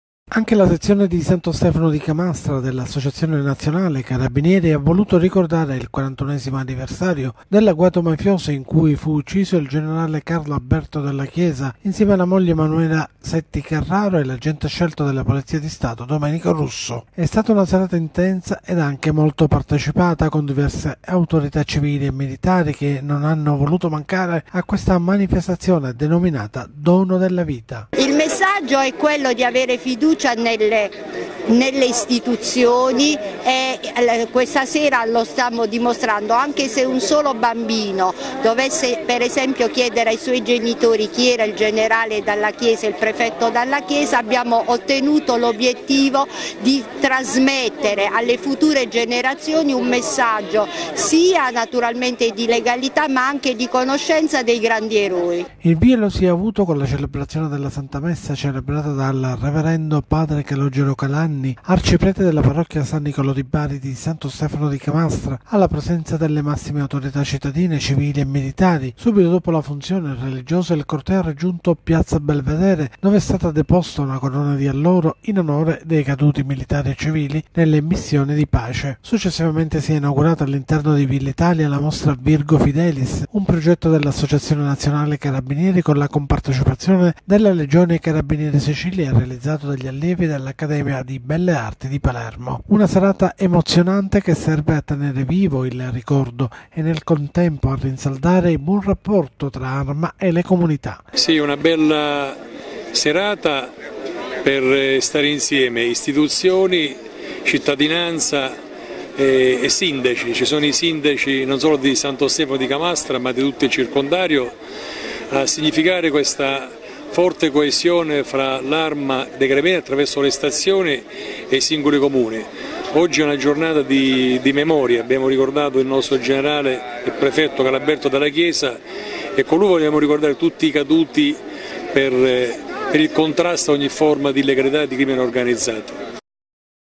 Santo Stefano di Camastra ricorda il generale Carlo Alberto Dalla Chiesa
A Santo Stefano di Camastra l'evento "Dono alla Vita" organizzato dalla locale associazione nazionale carabinieri con cui si è voluta ricordare la figura del generale Dalla Chiesa a 41 anni dalla sua morte per mano mafiosa